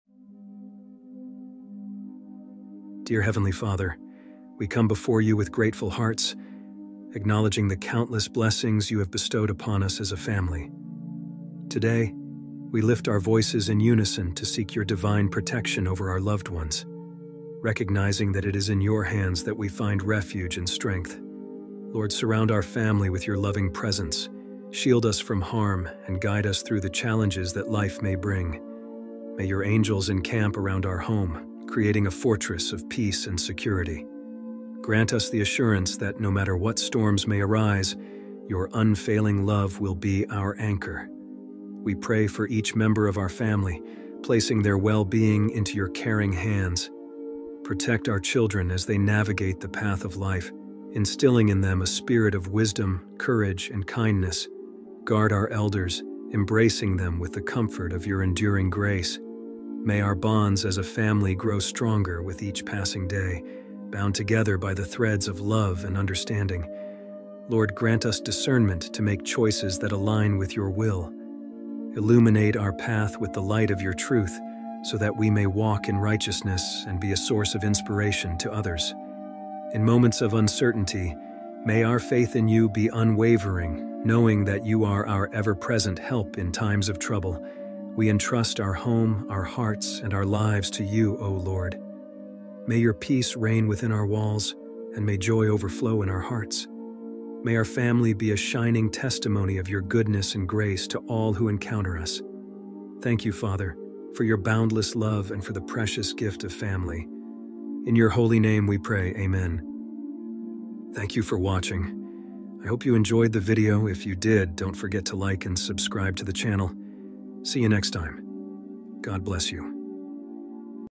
A Short Prayer To God For My Family - Lord God, Grant us the assurance that, no matter what...
Dear Heavenly Father, we come before you with grateful hearts, acknowledging the countless blessings you have bestowed upon us as a family. Today, we lift our voices in unison to seek your divine protection over our loved ones, recognizing that it is in your hands that we find refuge and strength. Lord, surround our family with your loving presence. Shield us from harm and guide us through the challenges that life may bring. May your angels encamp around our home, creating a fortress of peace and security. Grant us the assurance that, no matter what storms may arise, your unfailing love will be our anchor. We pray for each member of our family, placing their well-being into your caring hands. Protect our children as they navigate the path of life, instilling in them a spirit of wisdom, courage, and kindness. Guard our elders, embracing them with the comfort of your enduring grace. May our bonds as a family grow stronger with each passing day, bound together by the threads of love and understanding. Lord, grant us discernment to make choices that align with your will. Illuminate our path with the light of your truth, so that we may walk in righteousness and be a source of inspiration to others. In moments of uncertainty, may our faith in you be unwavering, knowing that you are our ever-present help in times of trouble. We entrust our home, our hearts, and our lives to you, O Lord. May your peace reign within our walls, and may joy overflow in our hearts. May our family be a shining testimony of your goodness and grace to all who encounter us. Thank you, Father, for your boundless love and for the precious gift of family. In your holy name, we pray, amen